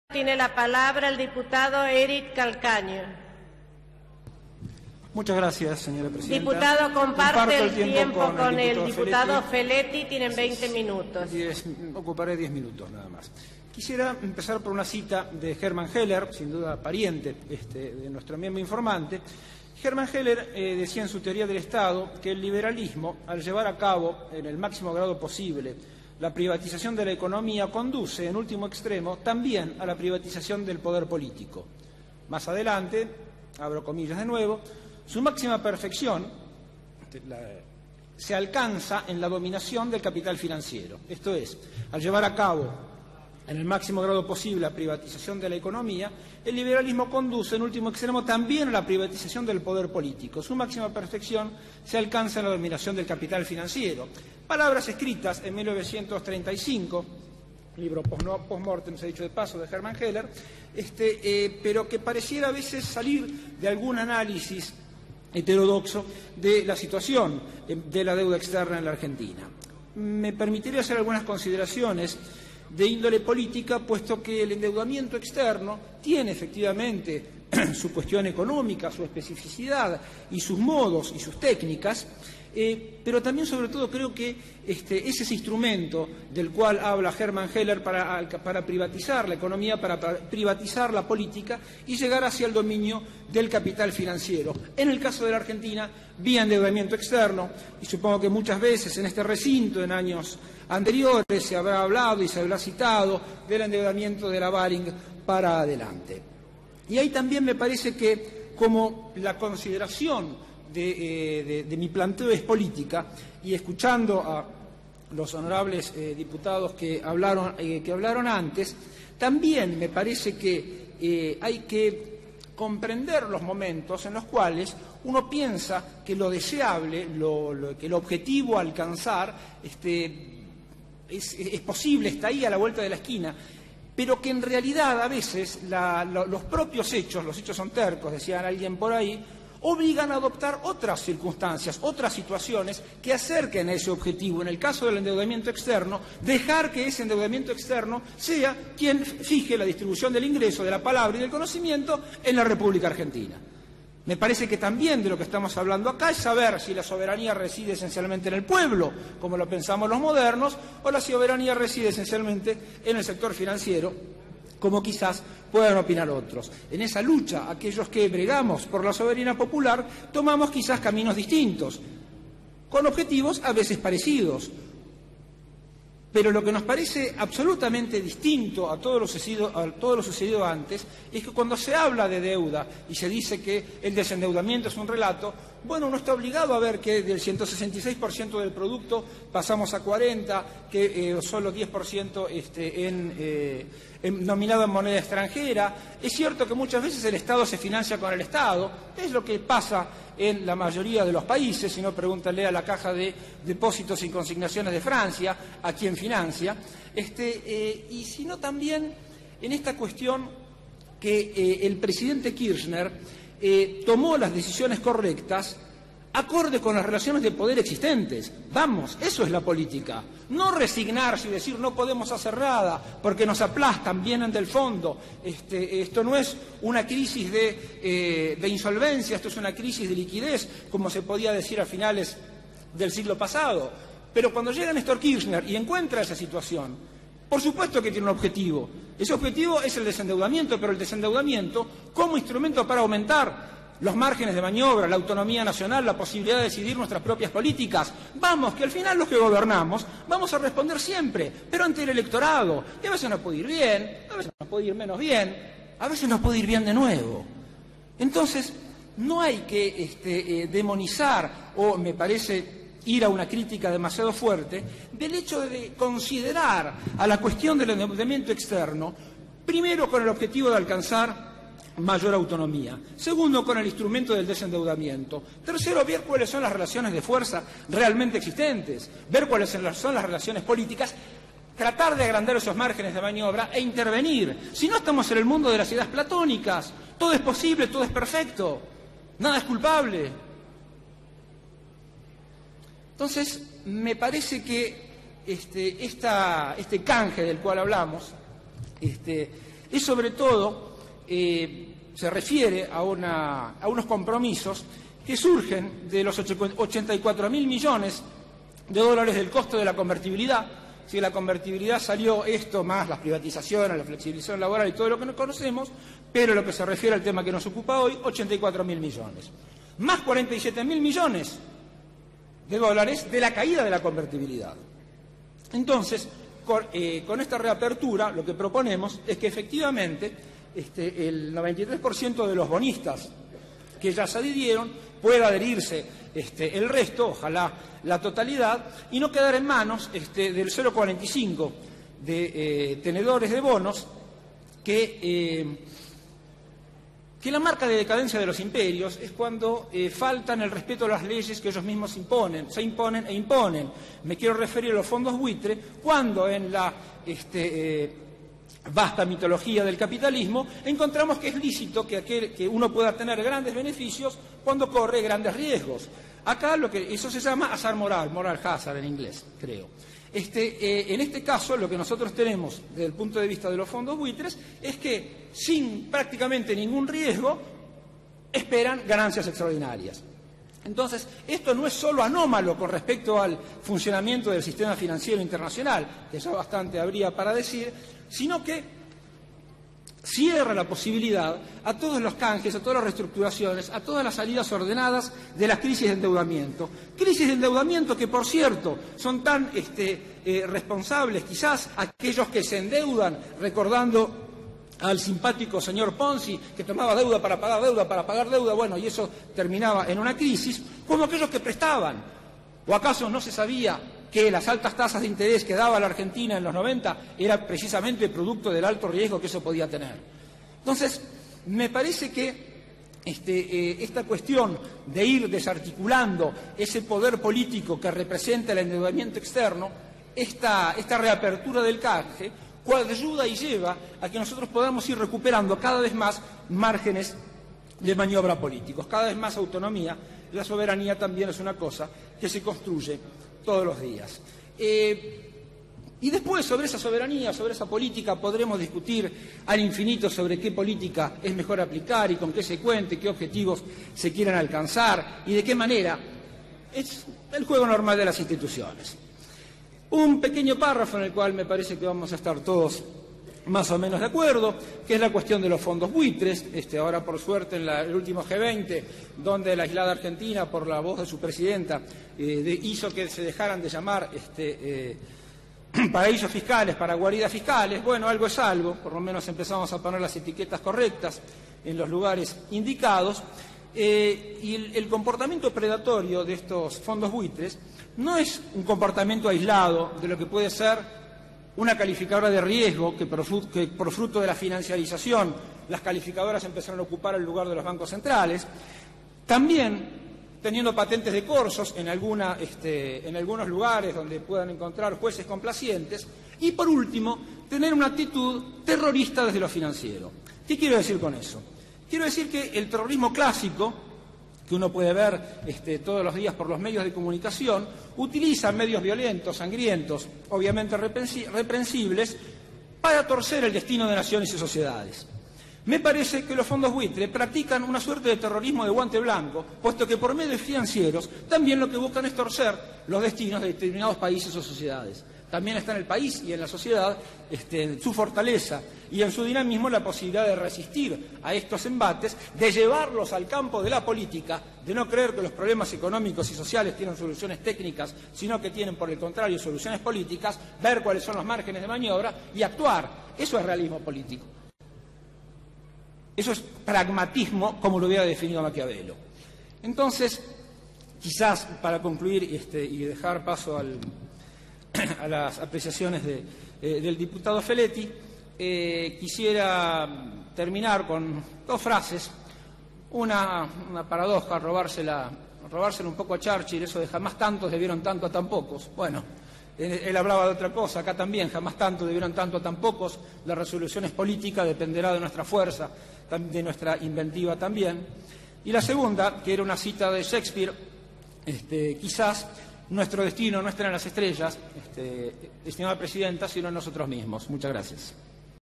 Eric Calcagno - HCDN - Reapertura canje de deuda - 9/11/2013
Discurso del diputado nacional Eric Calcagno en la Honorable Cámara de Diputados de la Nación sobre la Reapertura del Canje de Deuda